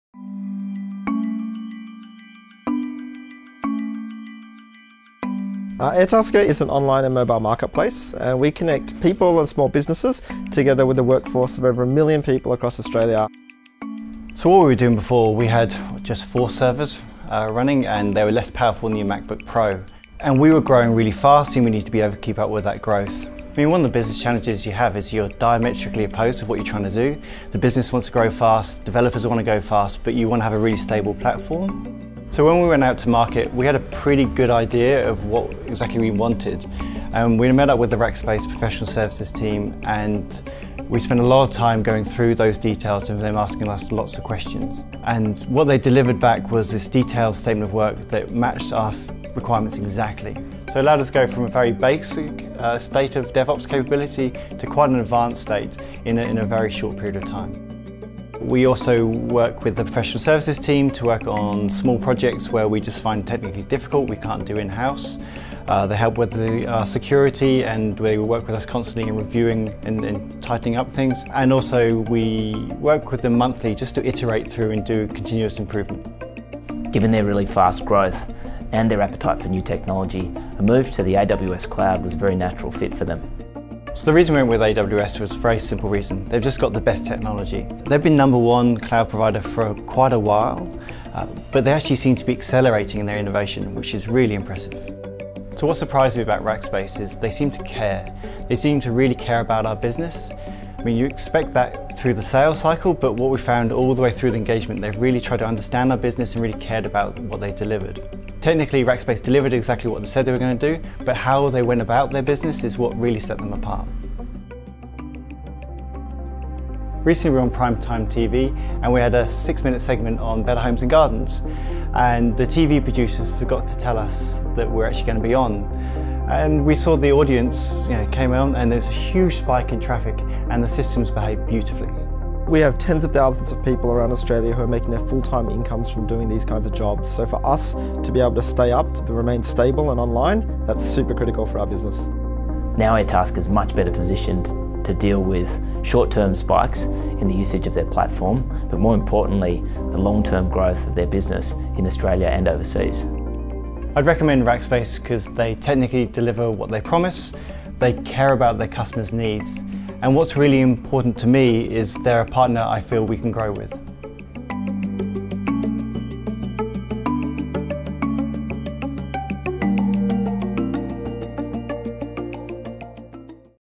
Rackspace Customer Stories: Online job marketplace Airtasker migrates to AWS
See how Rackspace® Fanatical Support® for AWS helped Airtasker scale automatically and seamlessly during intensive peak periods.

Airtasker provides an online and mobile job marketplace where users offer up a task and indicate a budget they are willing to pay for completion, community members then bid to complete the task. In this video Paul Keen, Airtasker CTO, discusses the need to meet the demands of their rapid growth and the benefits of moving to a stable AWS environment with Rackspace.